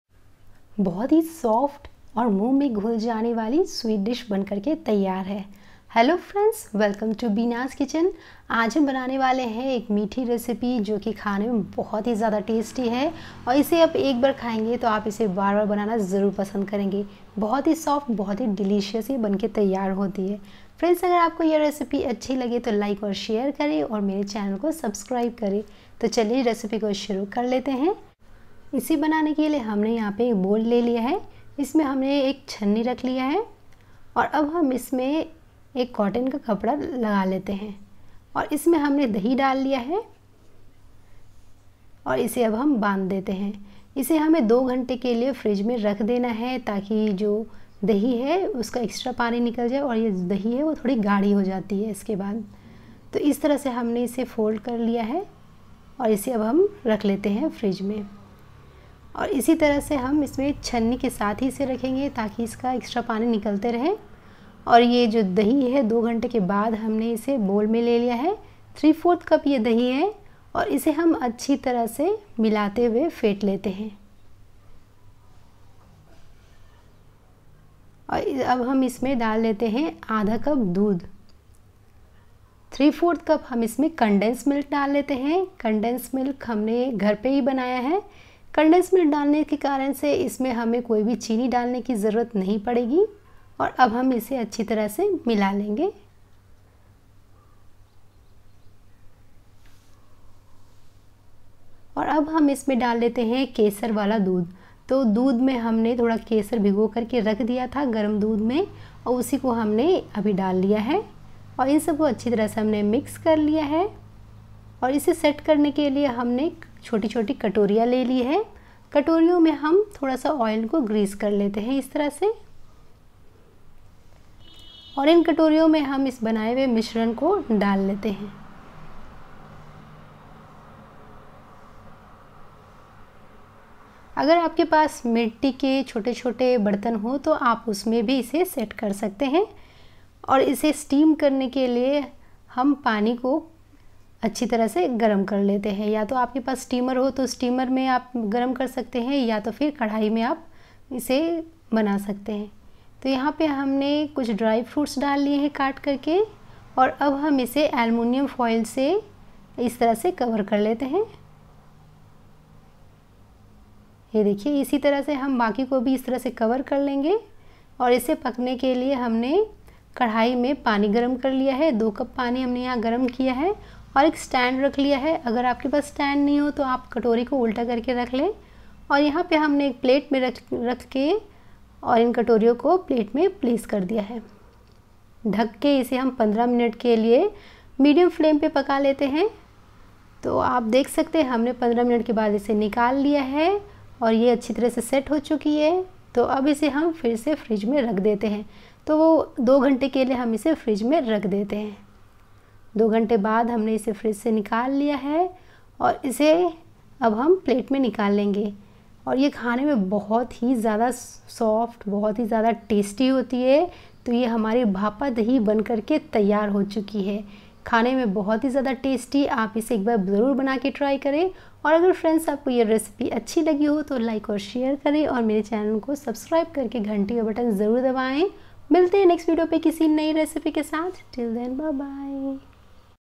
Lockdown में जब मीठा खाने का हो मन तो बनाये मजेदार Recipe I Recipe with 3 ingredients I bhapa doi
Sweet recipe I Bhapa Doi I Bengali Sweet I  Recipe with 3 ingredients I  bhapa doi Recipe